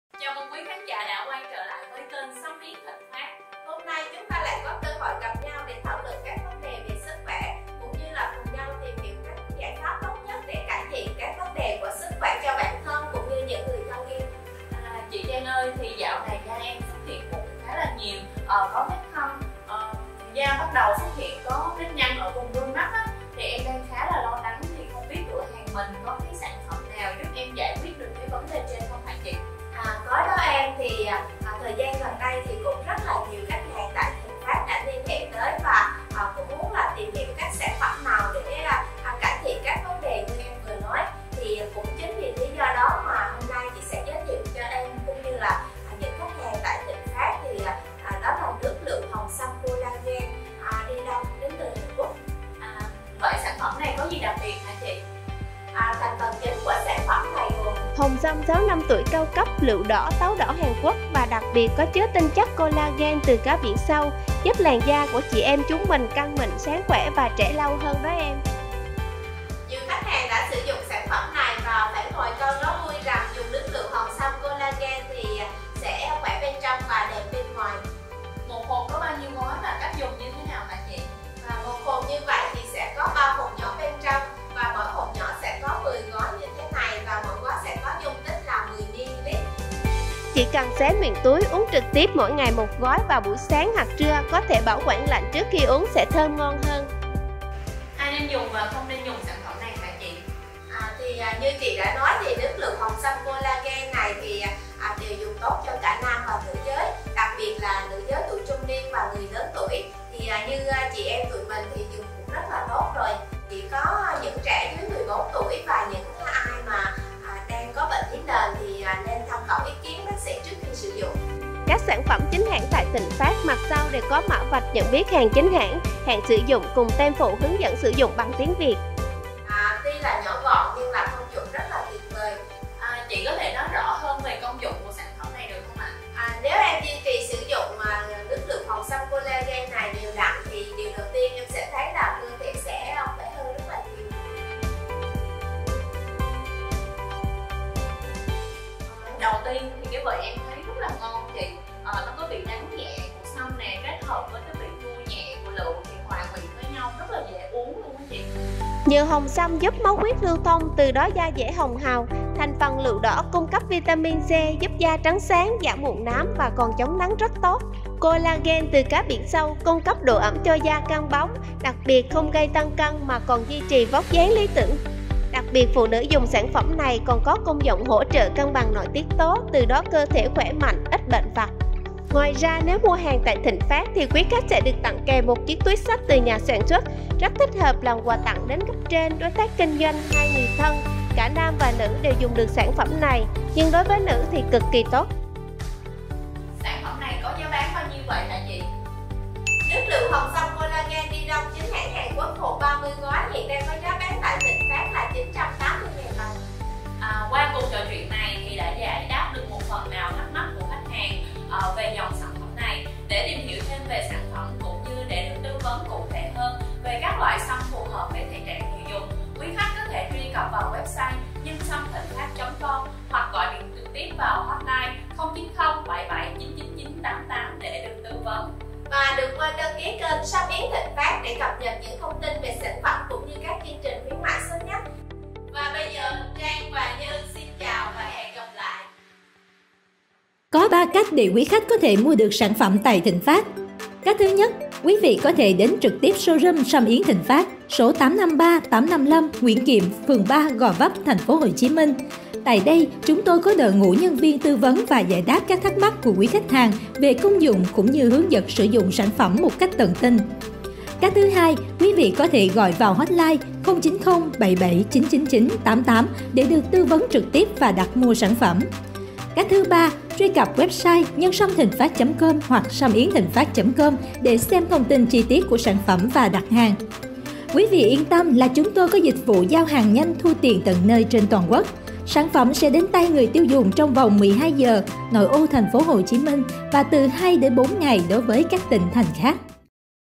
Cải Thiện Làn Da, Vóc Dáng Lý Tưởng Với Nước Lựu Hồng Sâm Collagen Daedong
🔥💥 Với thành phần chính chiết xuất từ: hồng sâm 6 năm tuổi, lựu Hàn Quốc, Collagen biển sâu,... Nước_lựu_hồng_sâm_Collagen_Daedong chính là giải pháp cho làn da và vóc dáng hoàn hảo của phái đẹp.

👉 Sử dụng sản phẩm thường xuyên từ 1-2 gói mỗi ngày sẽ giúp da được cung cấp đủ nước, chiết xuất lựu giúp ngăn ngừa tia UV trong ánh nắng mặt trời, trong khi đó hồng sâm giúp máu huyết lưu thông, hỗ trợ ngăn ngừa sạm nám.

🎁❤️ Đây còn là món quà lý tưởng mà cánh mày râu có thể mua tặng những người phụ nữ thương yêu.

🔎 MỌI GÓP Ý XIN GỬI VỀ:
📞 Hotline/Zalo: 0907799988